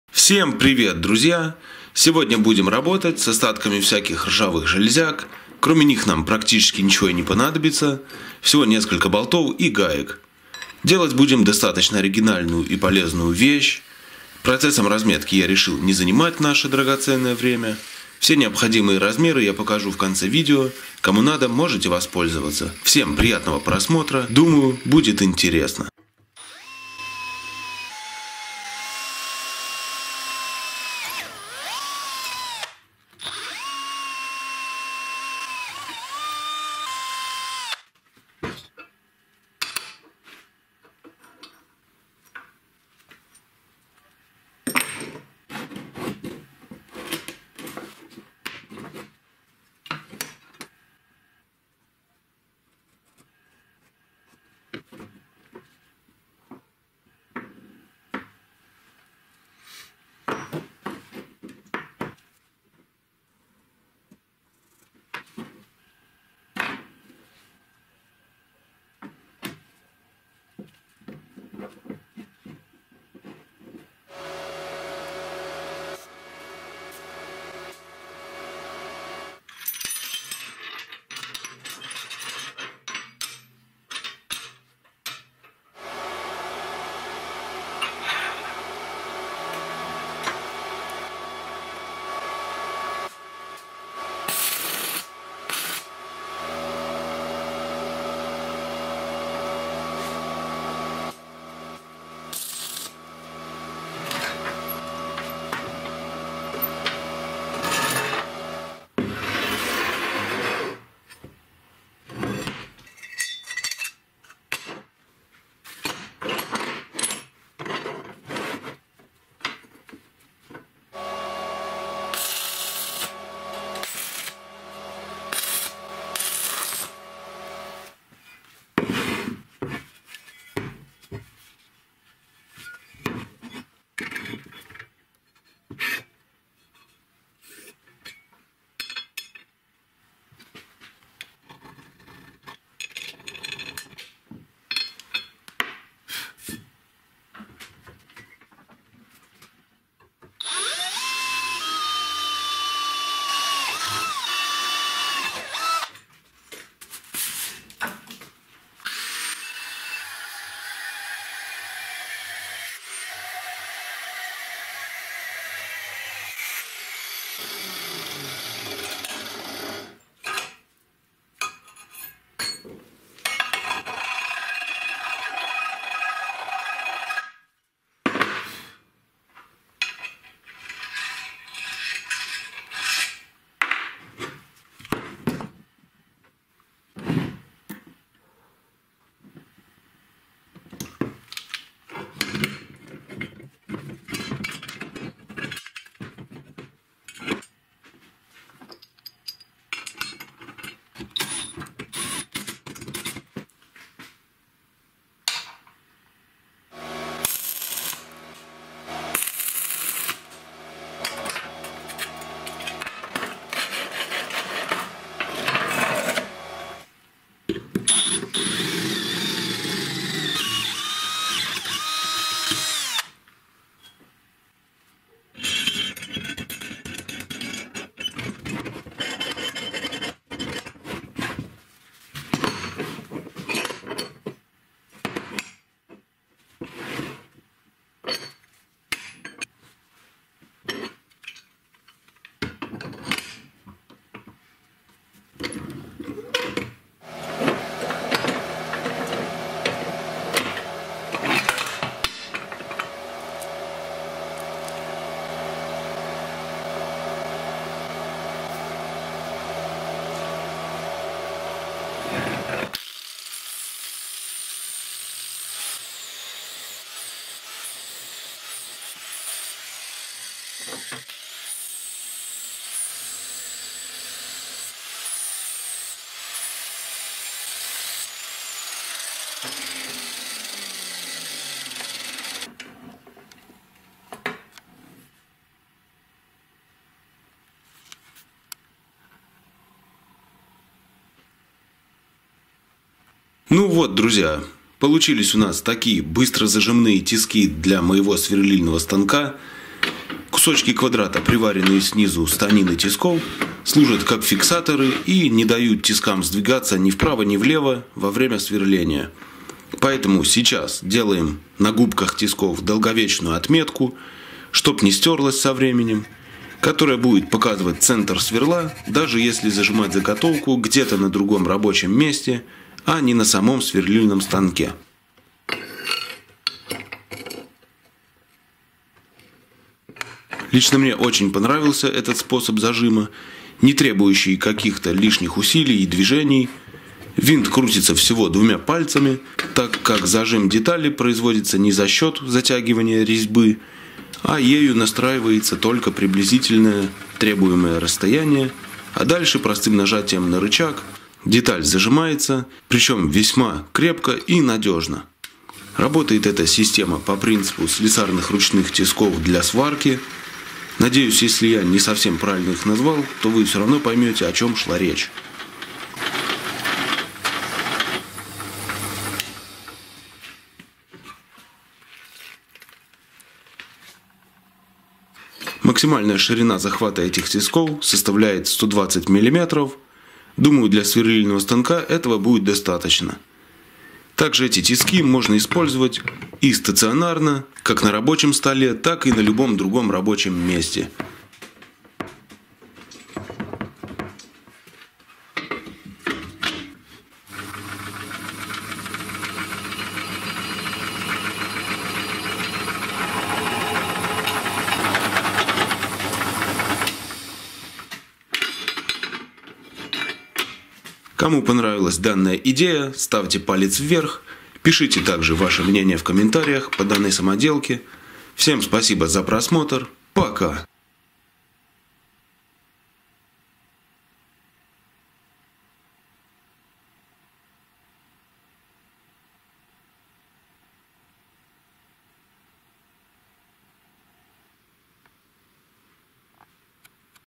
Настоящая мужская самоделка! Из ржавых кусков железа!
Всем привет! Меня зовут Тима.Я автор канала Workshop Diy.На своем канале делюсь идеями как из ненужных вещей делать полезные самоделки.В этом видео покажу идею из старых кусков железа!